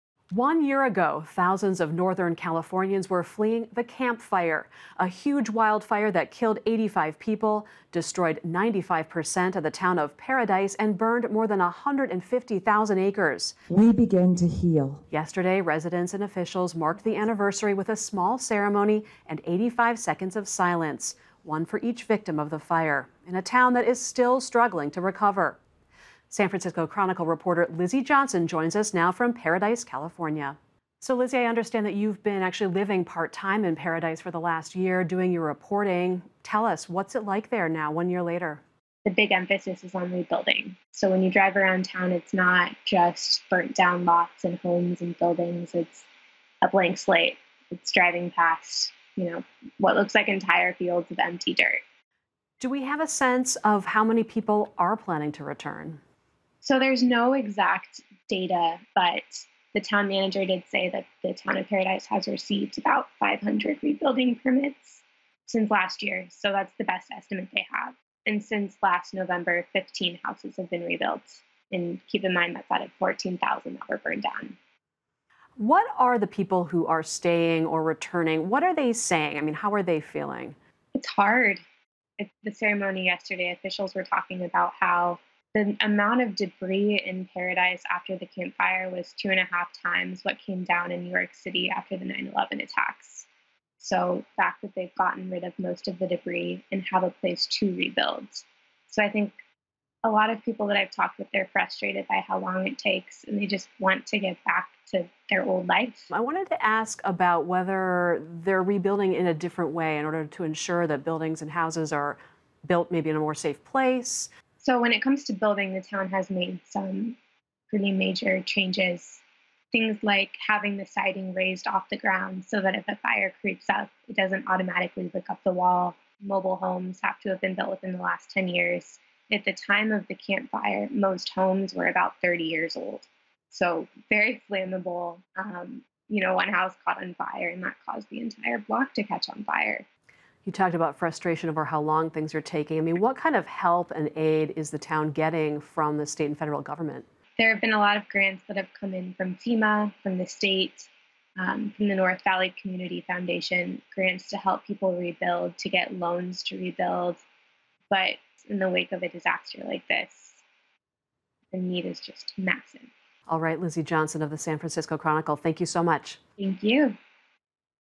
Paradise rebuilds a year after the Camp Fire
Residents and officials in Paradise, California, on Friday marked the one-year anniversary of the Camp Fire that devastated the Northern California community and killed 85 people. San Francisco Chronicle reporter Lizzie Johnson joins Megan Thompson for the latest on the rebuilding efforts and how the residents are coping.